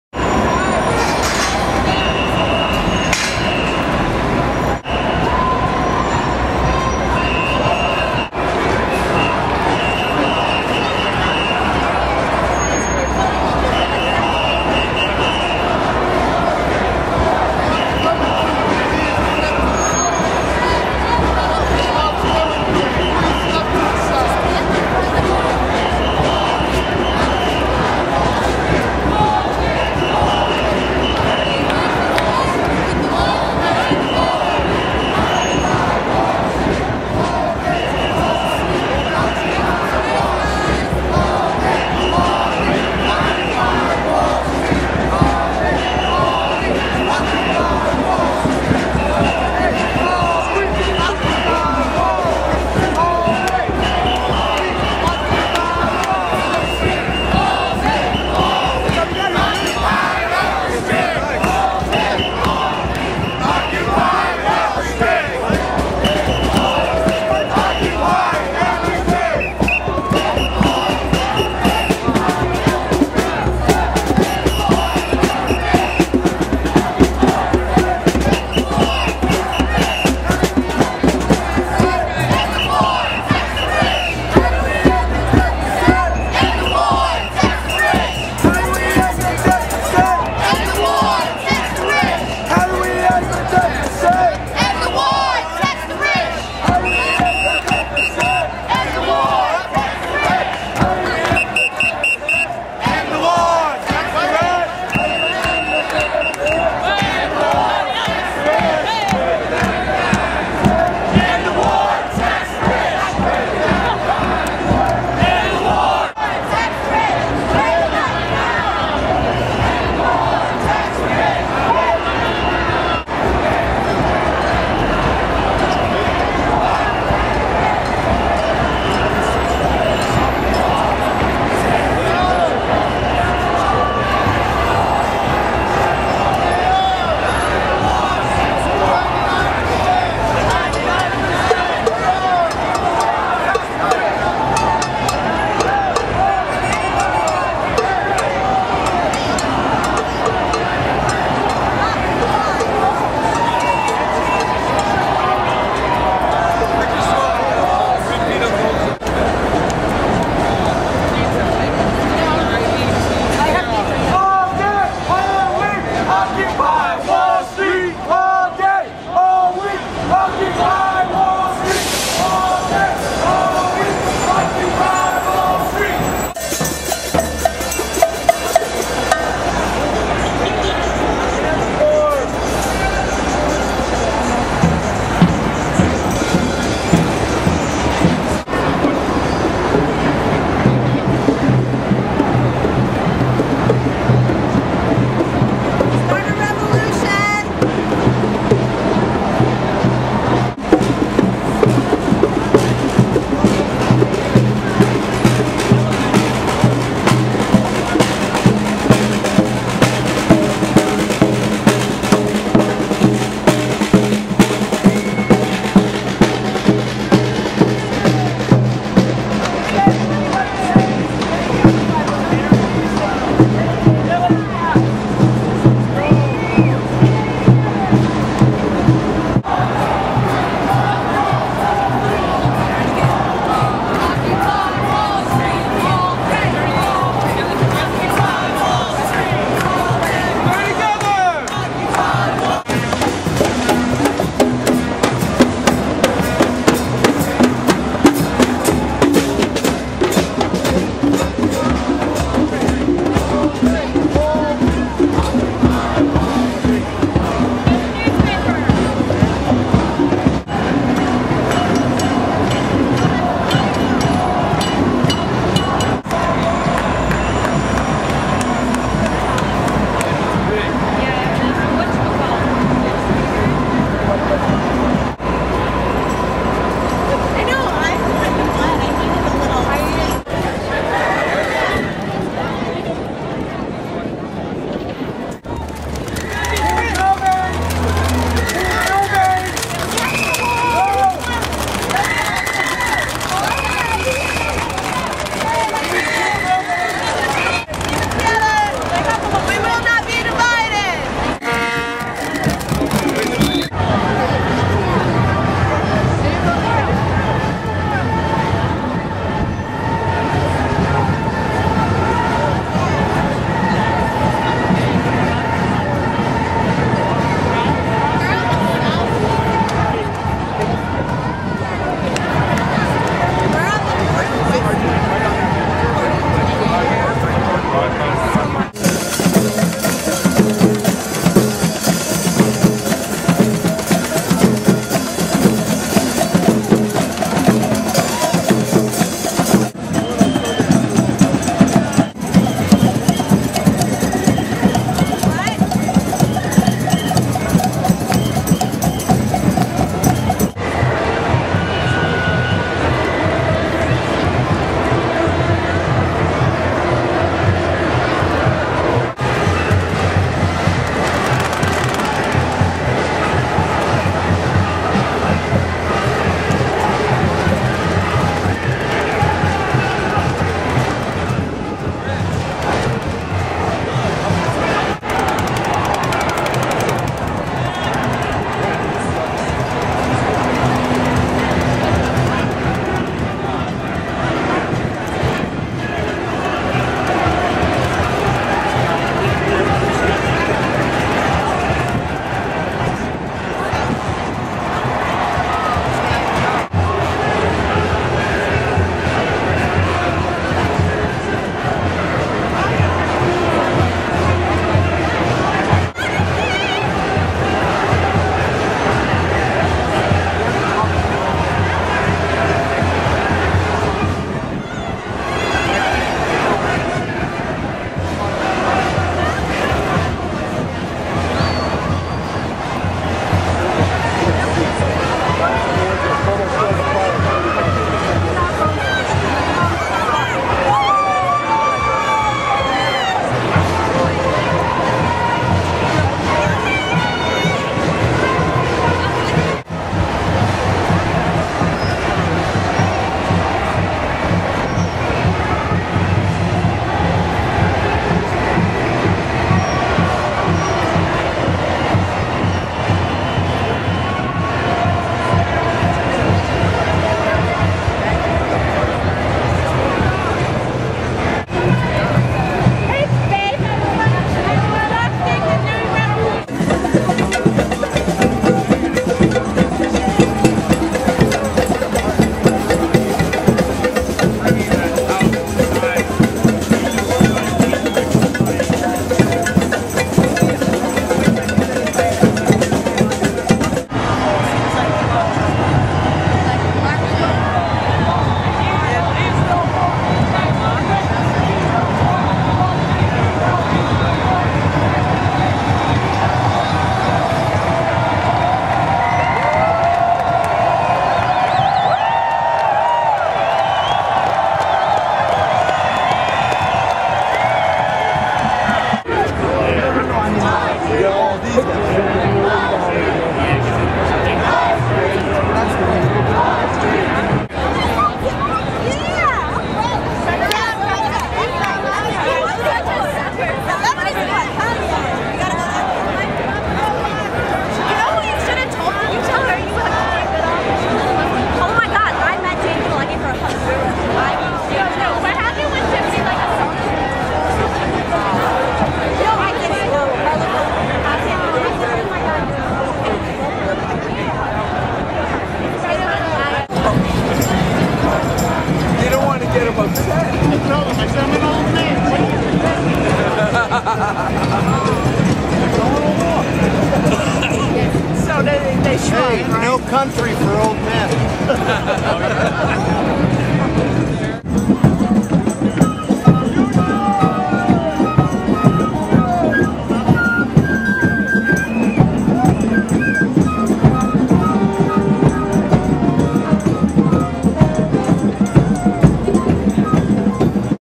Occupy Wall Street Oct. 5, 2011 March
From Zuccotti Park to Foley Square, in conjunction with labor unions.